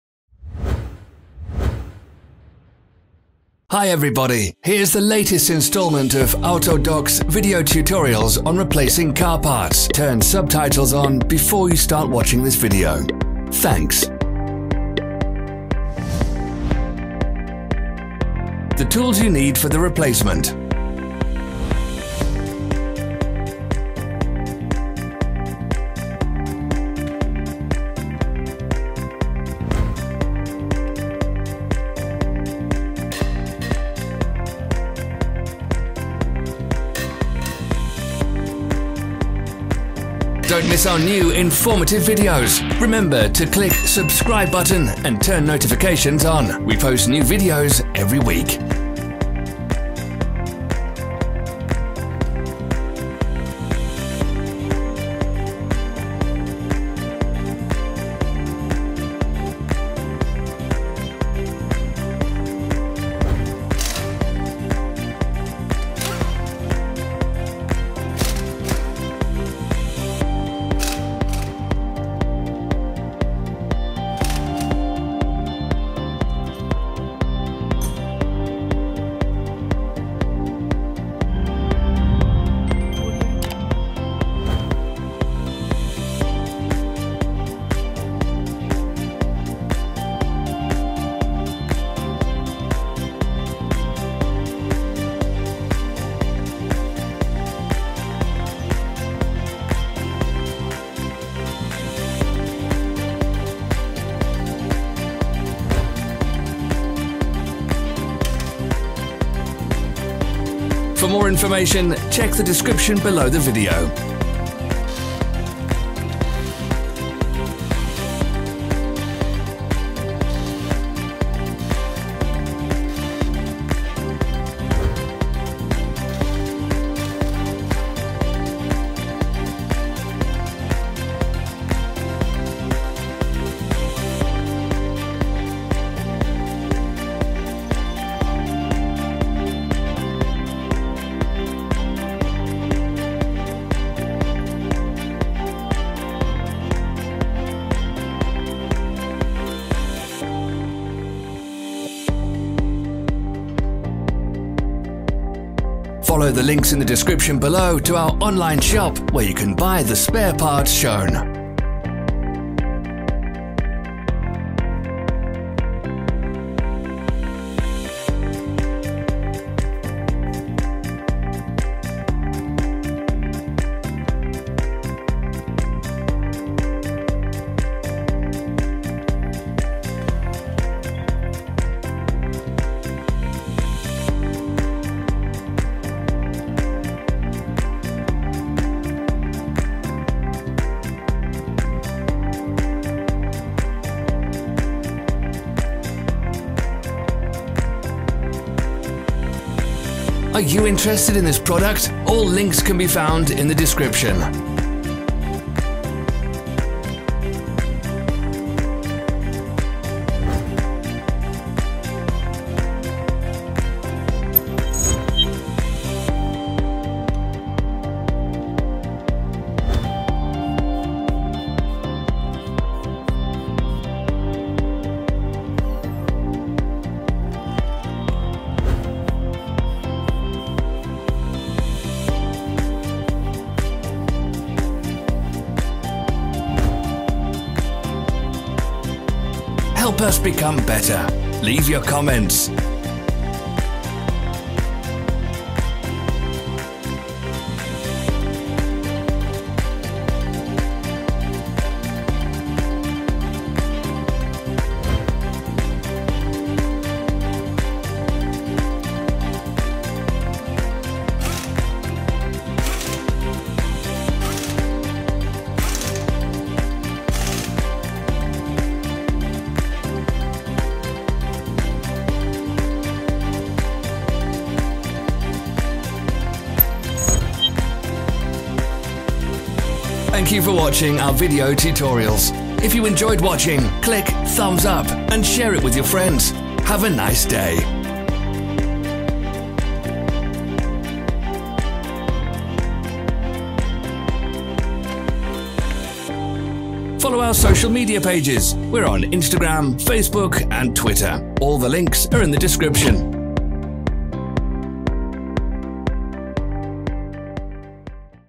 How to change rear springs / rear coil springs on VW PASSAT 5 (B5) [TUTORIAL AUTODOC]
How to change rear springs / rear coil springs / rear suspension springs on VW PASSAT 5 (B5) 1.9 Estate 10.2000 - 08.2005 [TUTORIAL AUTODOC]

Springs on AUDI A4 B5 (8D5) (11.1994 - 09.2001) / SKODA Superb I Saloon (3U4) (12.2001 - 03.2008) / AUDI A4 B6/B7 Convertible (8H7, 8HE) (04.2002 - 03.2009) / AUDI A4 B6 Avant (8E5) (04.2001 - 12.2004) / AUDI A6 C5 Avant (4B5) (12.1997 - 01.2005) cars should be replaced, as a rule, according to one and the same procedure.

🔝 Top brands that produce springs: STARK; RIDEX; KYB; MAPCO; JP GROUP.

All video and pdf tutorials on VW PASSAT 5 parts replacement: download and watch at AUTODOC CLUB

🛠️ The tools you need to replace rear springs:
- wire brush
- WD-40 spray
- brake cleaner
- copper grease
- drive socket No.16
- combination spanner No.16
- wheel impact socket №17
- ratchet wrench
- torque wrench
- hydraulic transmission jack
- wheel chock

► How to replace front anti roll bar link / front drop link on VW PASSAT 5 (B5)